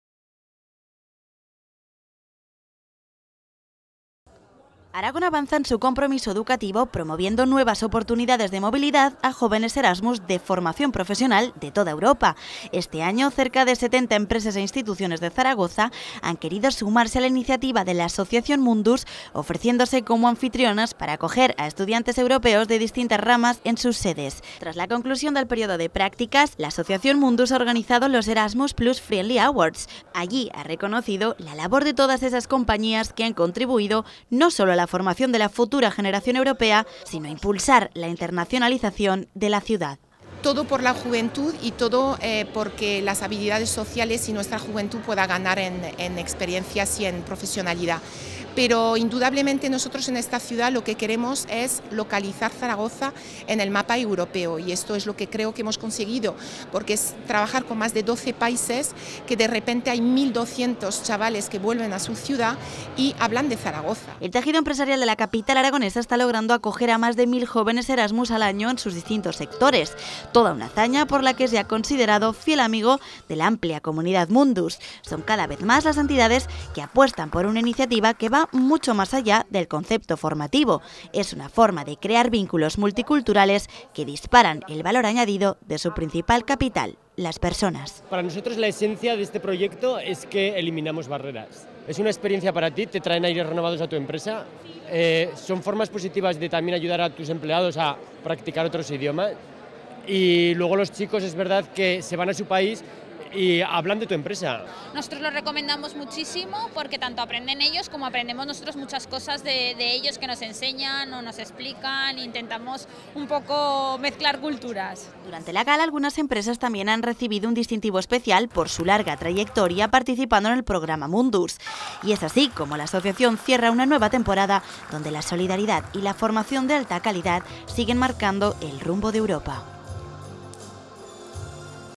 La Asociación Mundus reconoce a 70 empresas zaragozanas por acoger a estudiantes FP de toda Europa
Aragón avanza en su compromiso educativo promoviendo nuevas oportunidades de movilidad a jóvenes Erasmus de FP de toda Europa. Este año, cerca de 70 empresas e instituciones de Zaragoza han querido sumarse a la iniciativa que lleva impulsando la Asociación Mundus desde hace más de siete años, ofreciéndose como anfitrionas para acoger a estudiantes europeos de distintas ramas educativas durante varios meses en sus sedes. Tras la conclusión del periodo de prácticas, la Asociación ha organizado los Eramus+ Friendly Awards para reconocer públicamente la labor de todas esas compañías que han contribuido no solo a la formación de la futura generación europea, sino que han ayudado a impulsar la internacionalización de la ciudad.